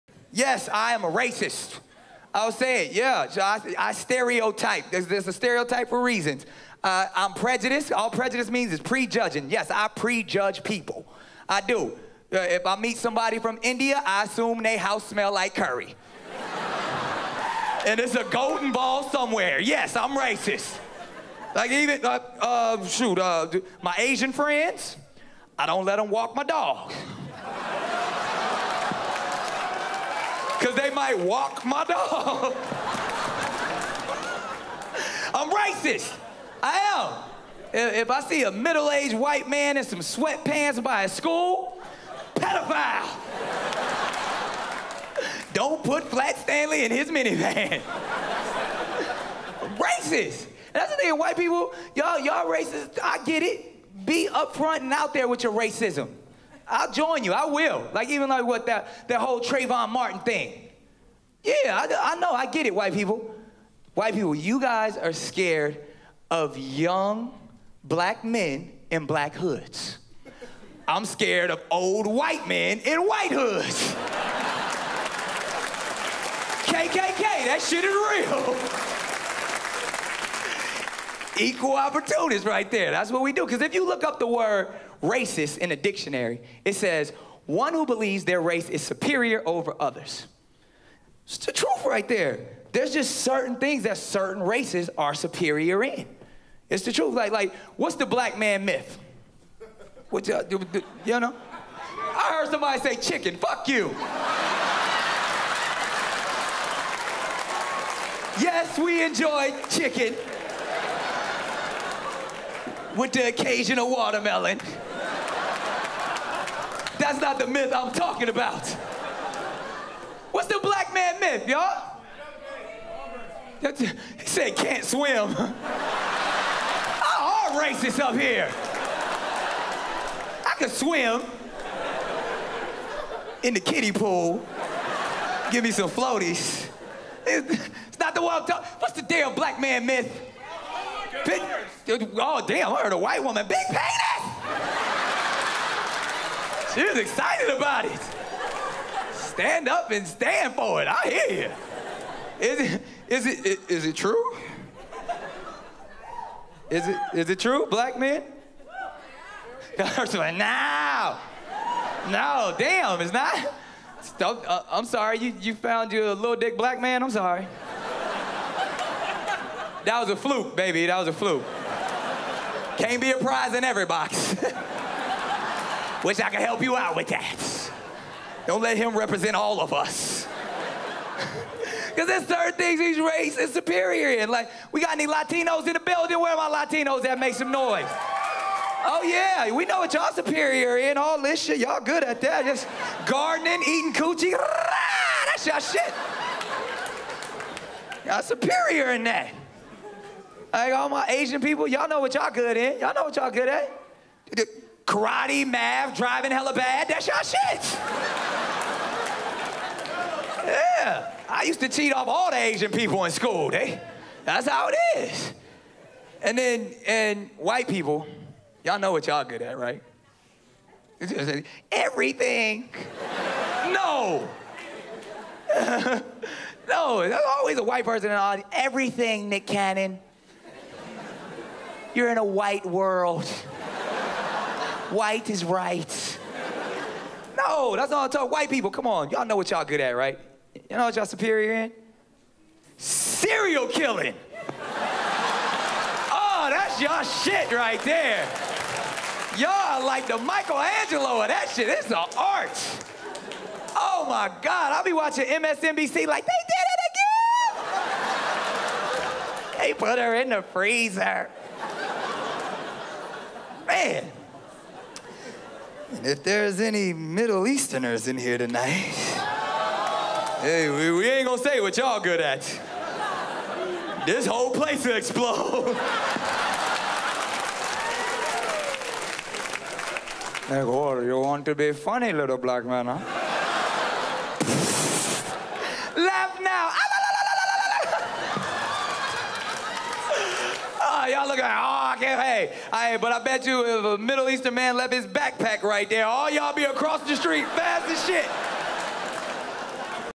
I'M RACIST (Full)
seems as though people thought i was being too tame with the previous clip...so here's a little more, complete with a few things EVERYONE thinks but is too afraid to say!